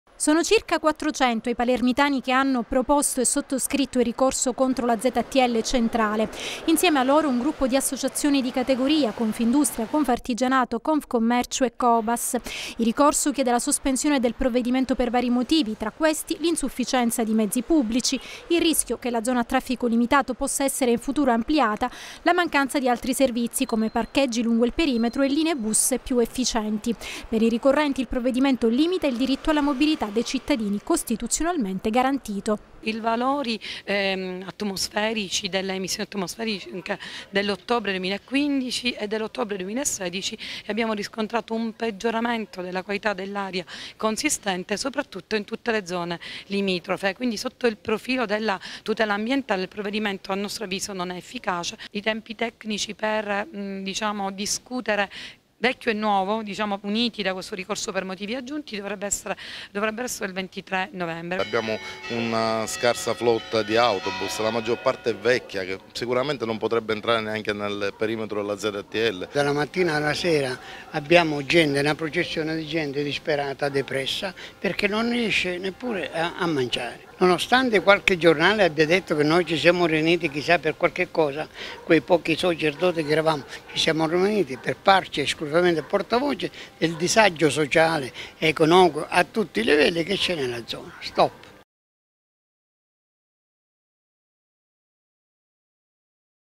Ztl, presentato nuovo ricorso pronte 400 firme [TgMed 07/11/2016]
C'erano anche dieci salme a bordo della nave che oggi ha trasferito al porto di Palermo 1.045 persone. Intanto a Pozzallo è stato fermato il presunto scafista di un'imbarcazione nella quale una donna del Mali è morta per fare scudo ai propri figli.